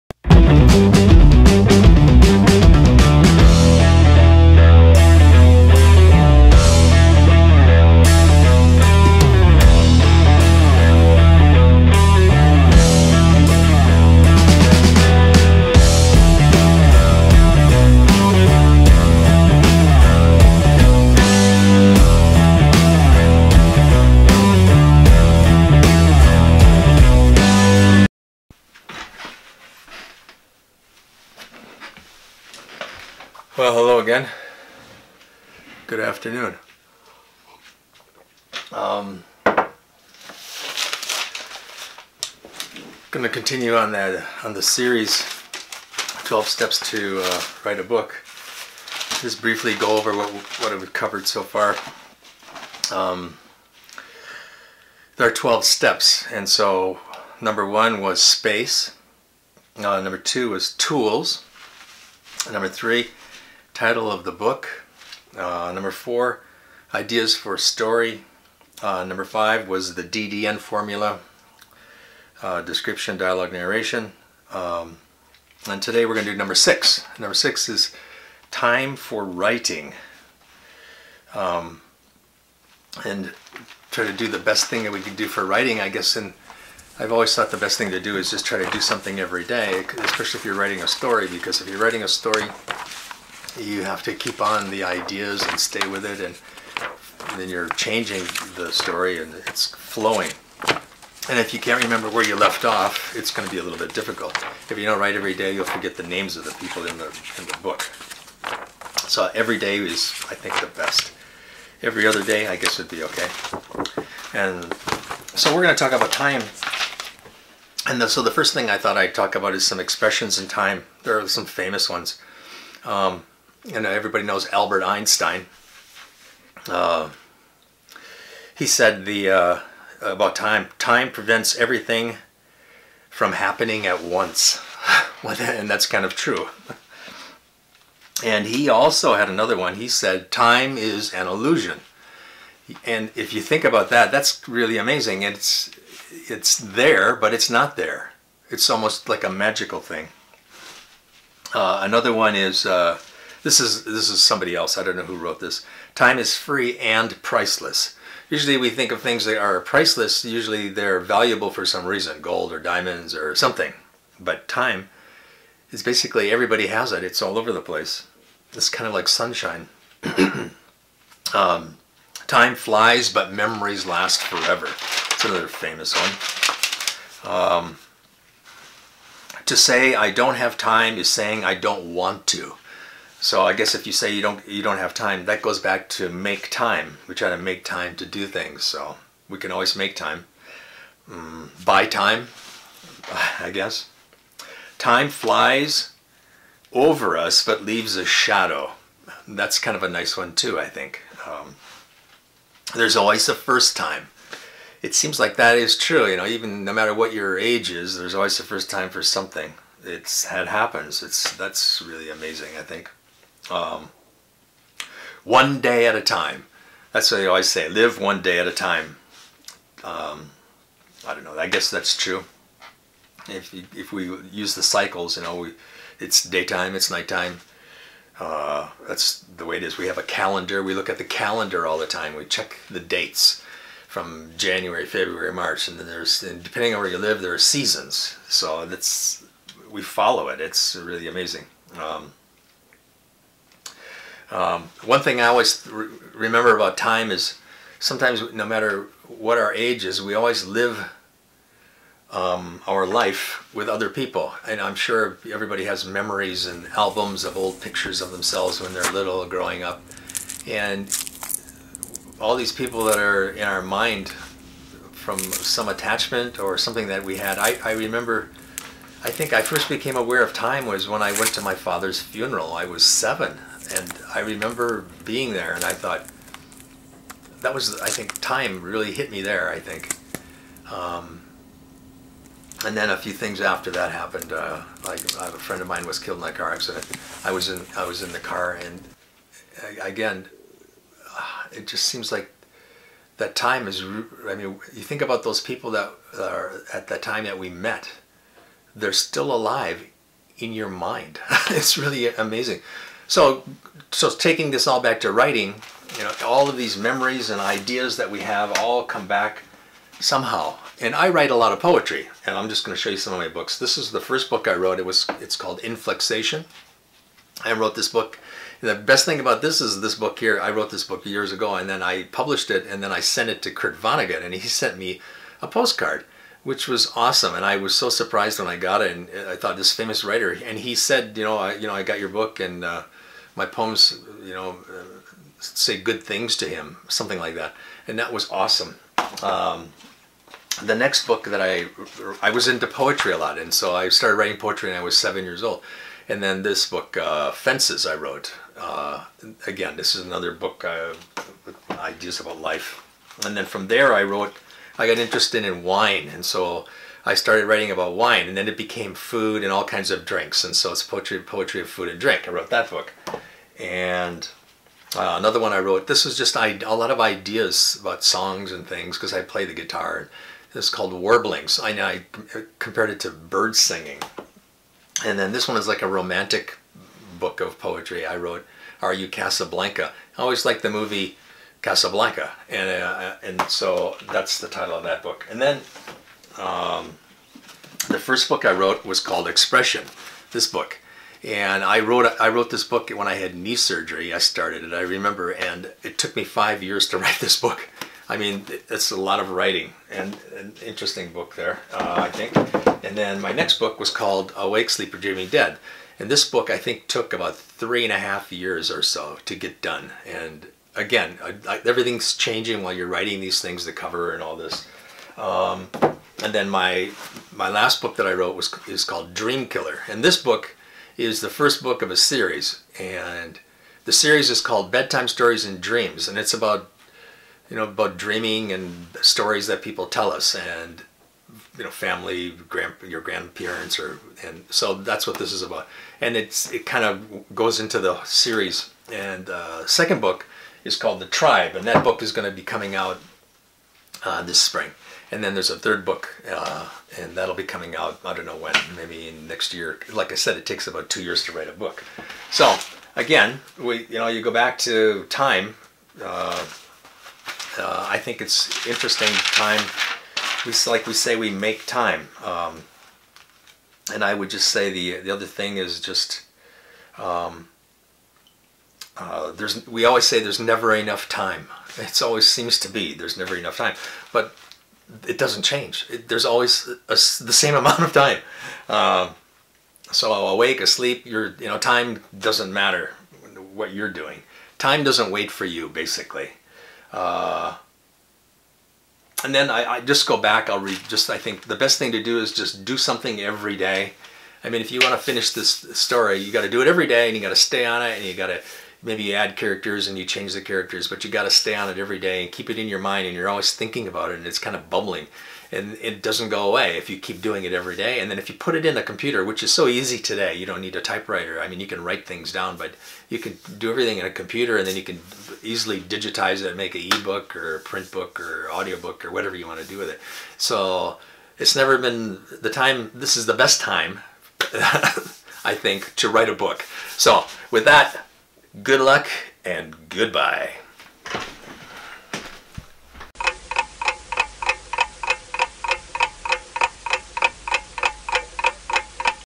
12 Steps to write a book - Step 6 - Making Time
This is the sixth video - Making Time - in my series 12 steps to write a book. I talk about how time is what we use or burn in our life. Everyone gets the same amount of time, but using it wisely is needed to do anything that takes time. Writing, editing, publishing a book takes a lot of time effort to do it well.